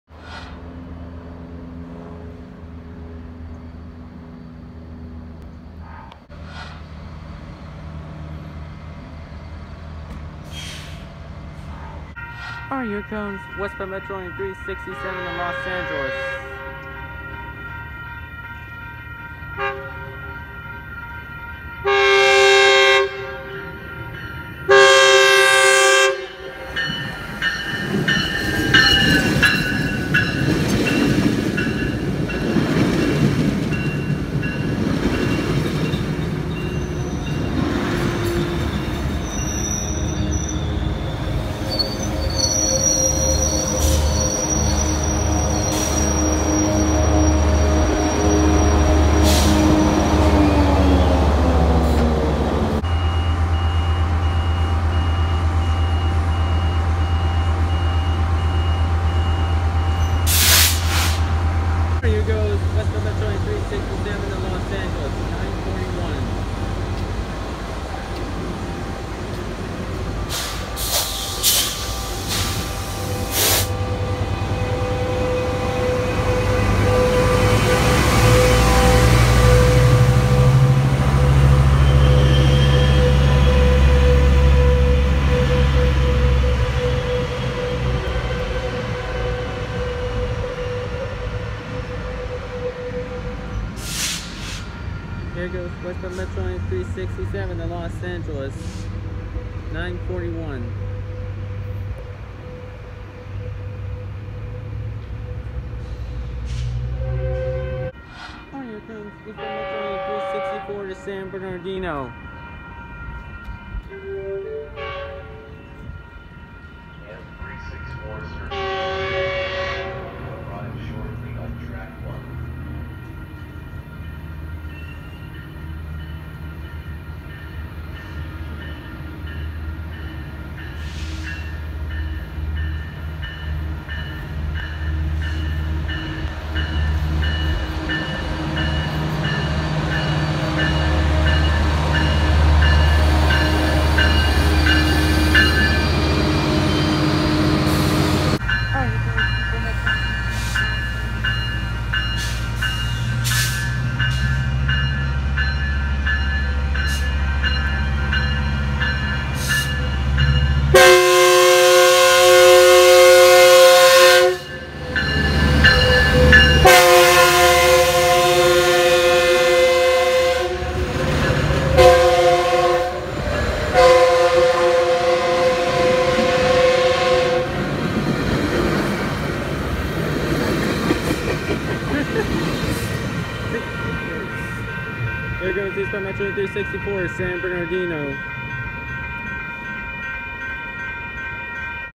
Railfanning In Baldwin Park On 3/6/21
Baldwin Park CA

On Saturday, March 6th 2021. I Had An Awesome Day While Railfanning At Baldwin Park And I Capture Some Of Our Metrolink Trains 367 @ 364 With An SB Lines. I Hope You Enjoy Watching This Video.

Trains

Train 1 Westbound Metrolink M367 Service To LA Union Station CABCAR 689, F125 941.

Train 2 Eastbound Metrolink M364 Service To San Bernardino F125 919, CABCAR 658.